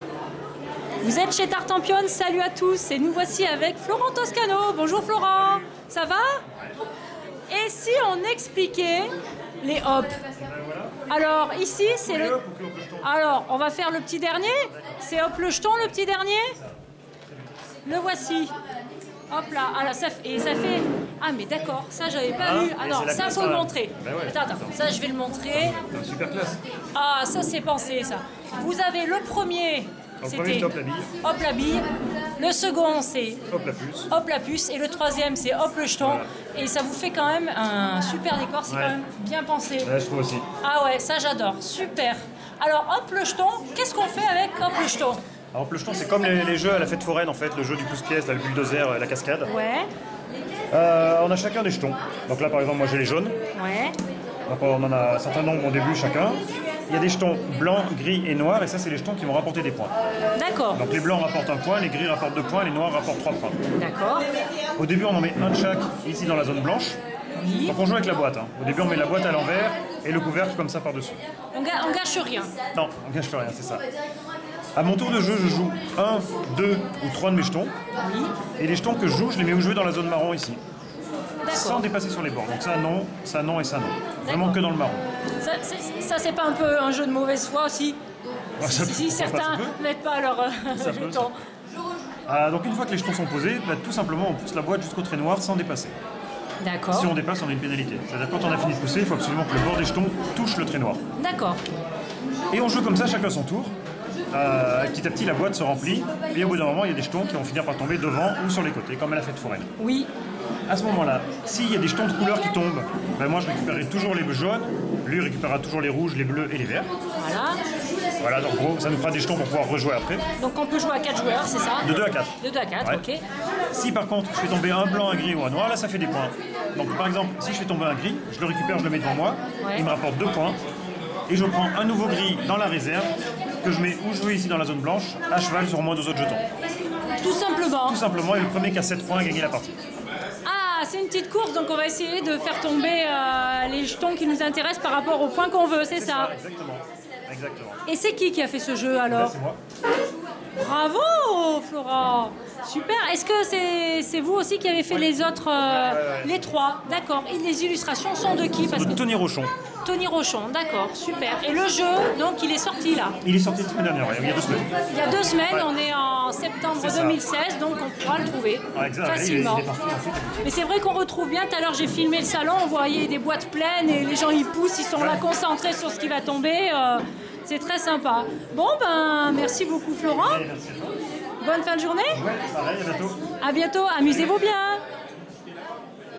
Hop le j'ton ! les règles du jeu
Florent Toscano, auteur des jeux de la série "Hop", nous présente le petit dernier. Amusez-vous bien !